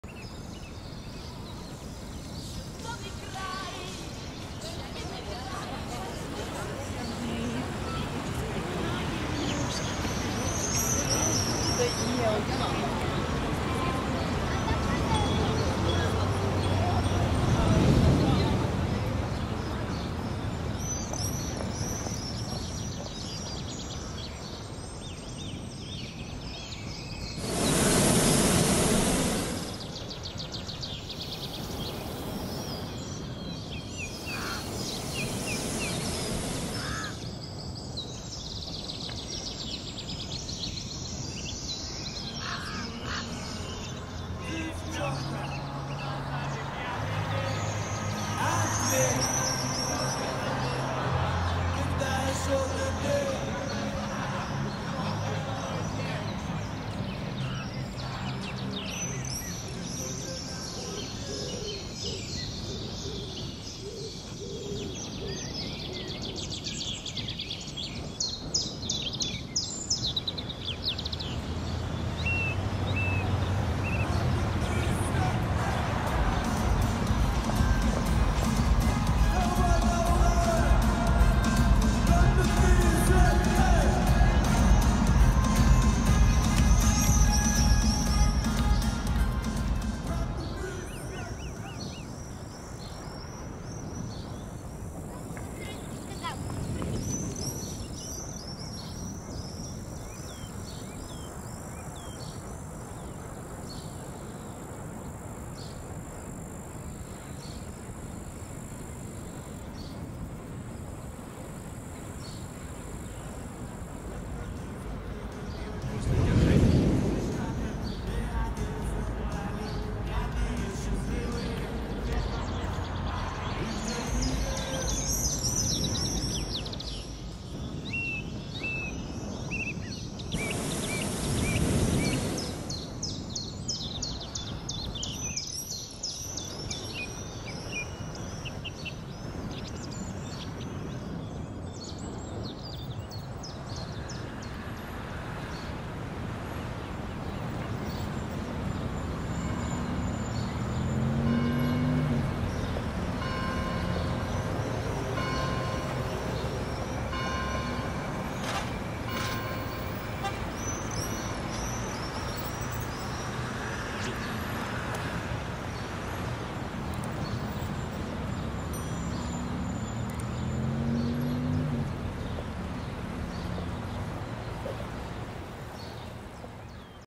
Камʼянець-Подільський / 4K Cinematic FPV
Полетал над замком и воздушными шарами в прекрасном городе Каменец-Подольский, окунитесь в атмосферу благодаря объёмному саунд-дизайну.